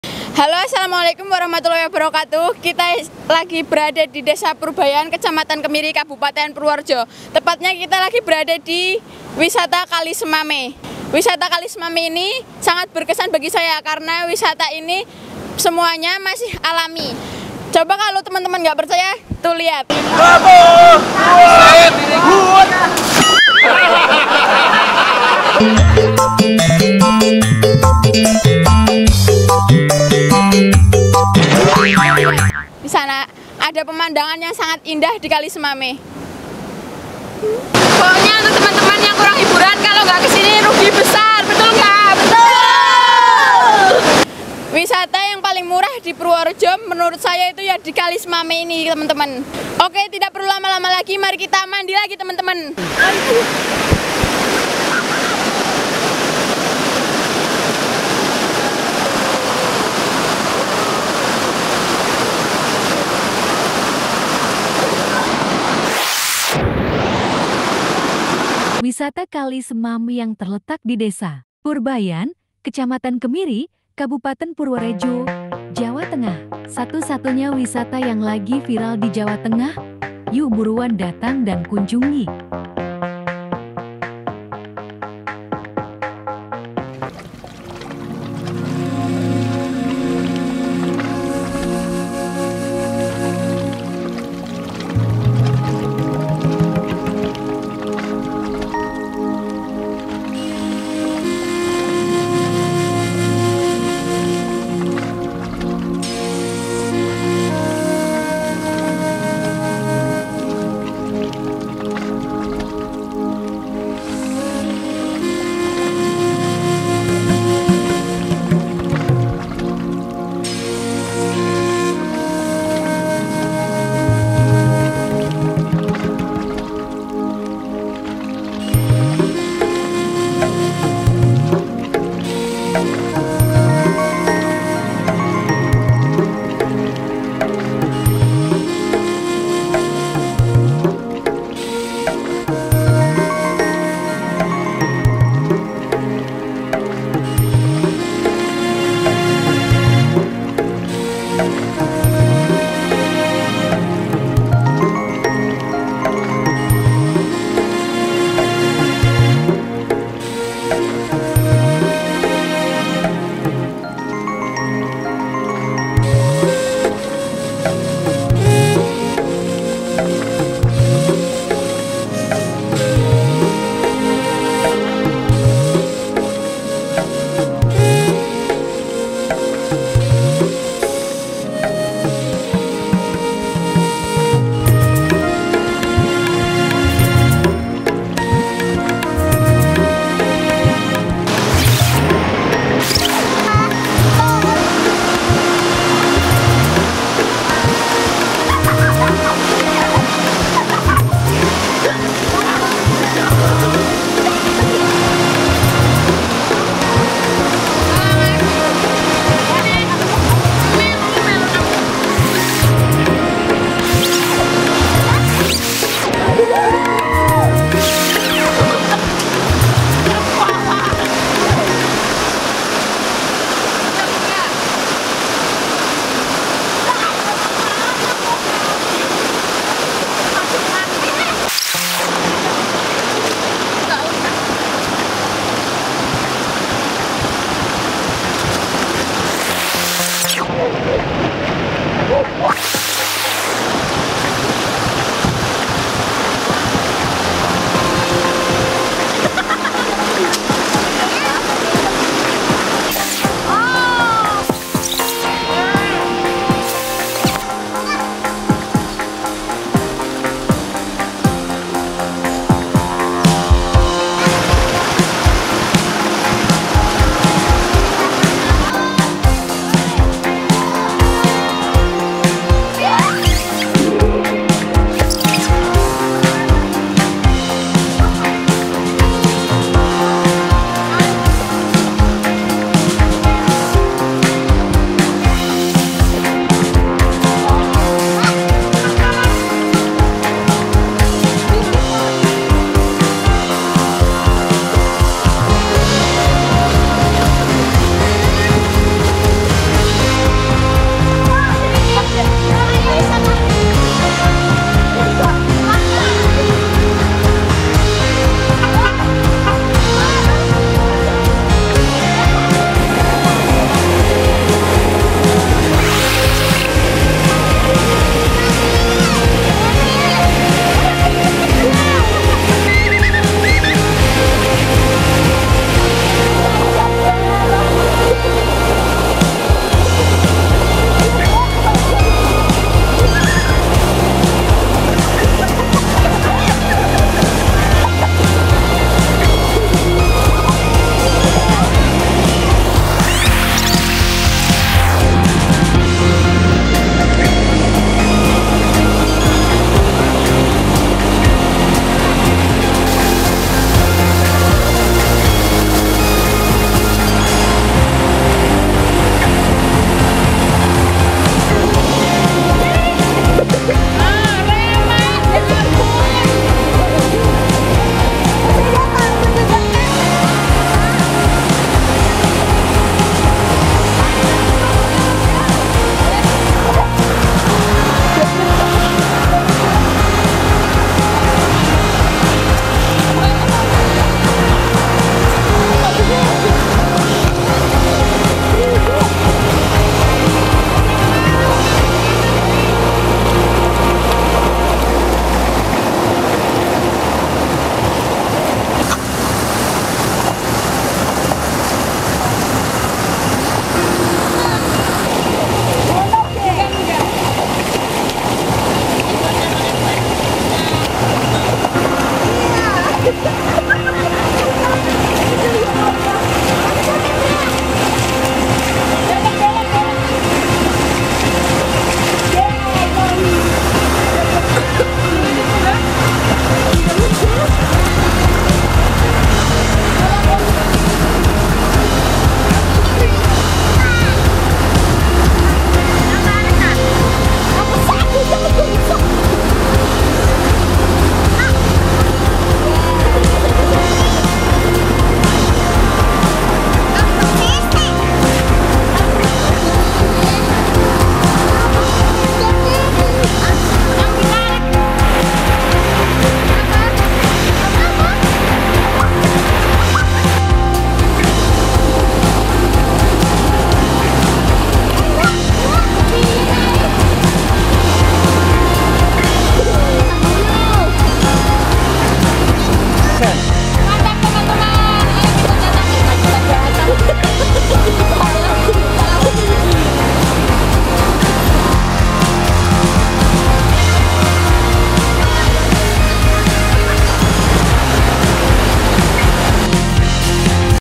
ARUNG JERAM /DI WISATA KALI SEMAME SUDAH TERSEDIA ARUNG JERAM
Arung Jeram, Arung Jeram Di Wisata Kali Semame Wisata kali Semame  Semua Serba Alami Dan Spot Foto Nya Keren Keren, Bagi Temen Temen Yg Mau Menyegarkan Fikiran yg Lagi Gabut Datang Aja Ke Wisata Kali Semame Desa Purbayan Kemiri Purworejo